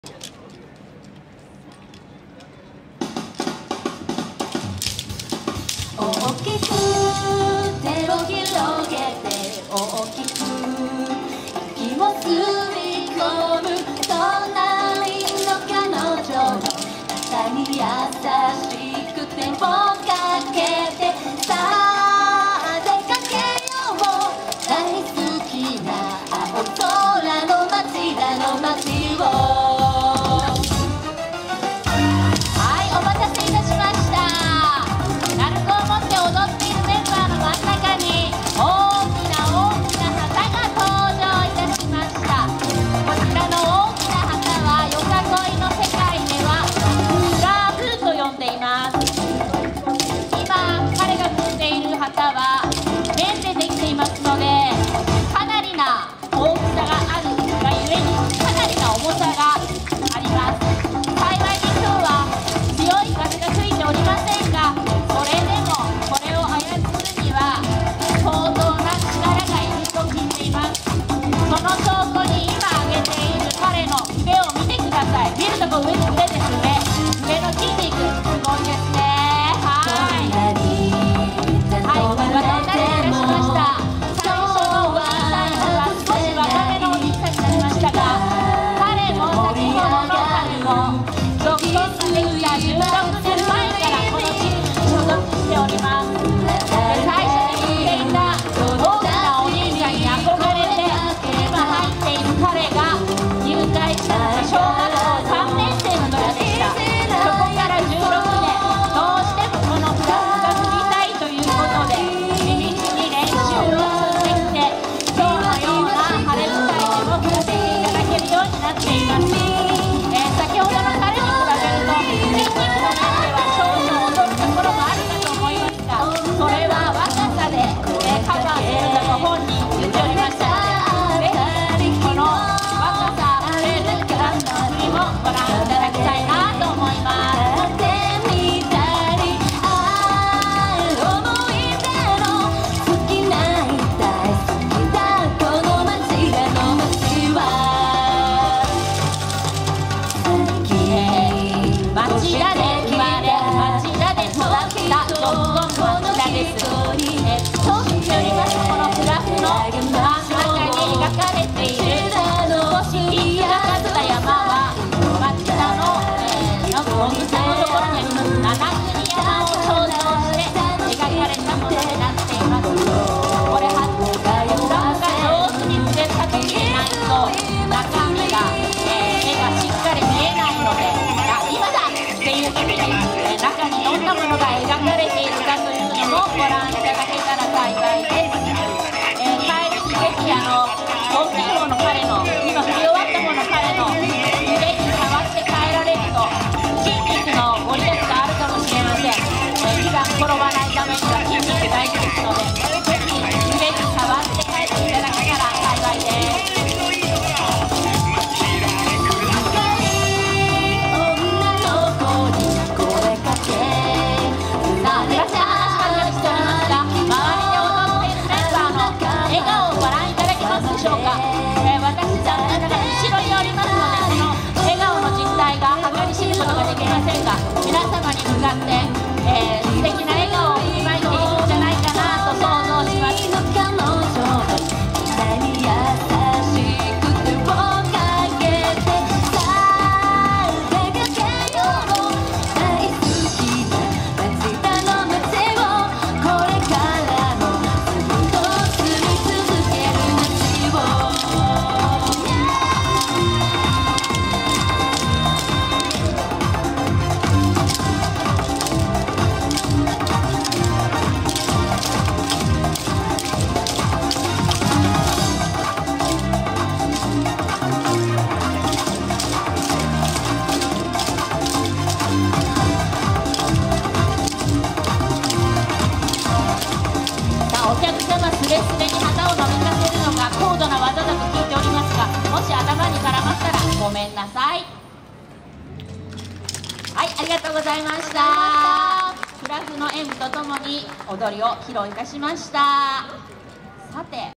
ぞっこん町田'98_演舞①_町田大道芸2014_141012
2014年10月11日・12日、町田駅周辺エリアで「町田大道芸2014」が開催されました。

ぞっこん町田'98 オフィシャルサイト